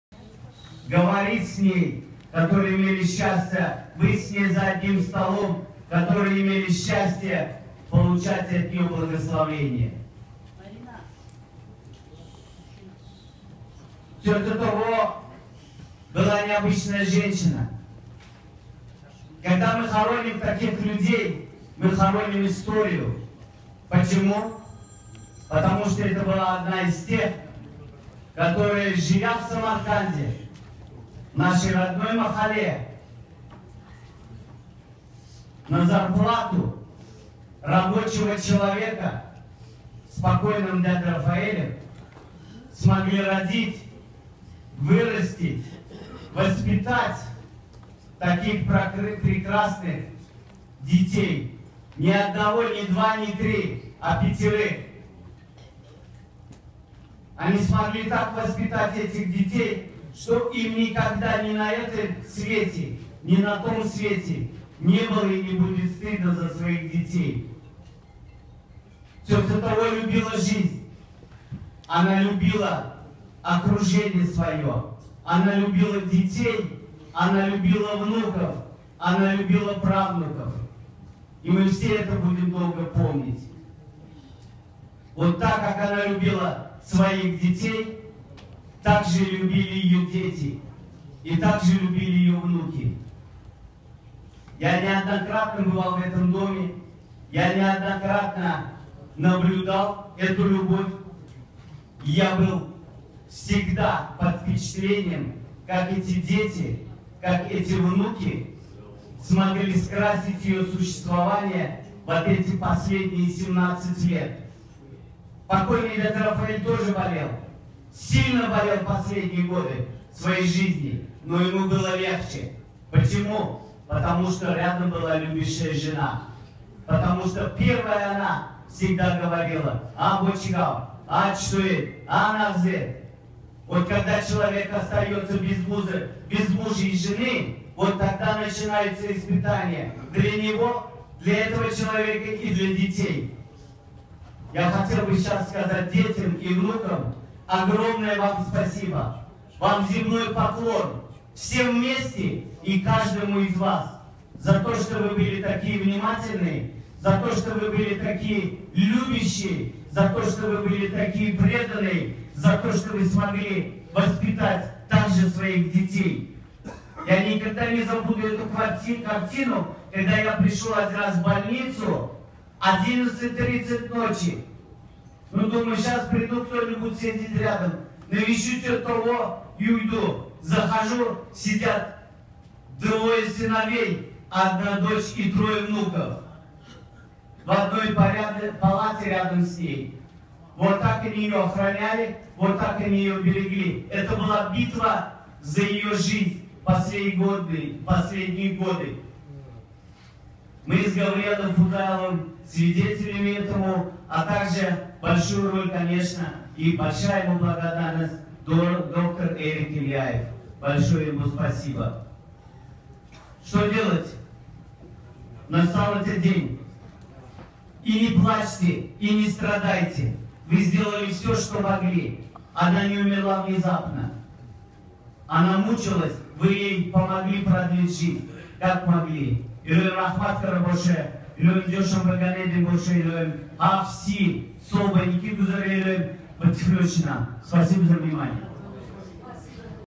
IMG 12941, Misha Normatov, Dr OLEG FUZAYLOV
POKHORONI LAVOYO IN LOVING MEMORY OF TOVA KANDKHOROV (AKILOV) BAT SORO AND NISON, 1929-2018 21 AdAR 5778 , ESHET RAFOEL, MOTHER SARA, ESYA, YOSEF, GRISHA , ILYOS. IN Shwartz Brothers Jeffer Memorial Chapel.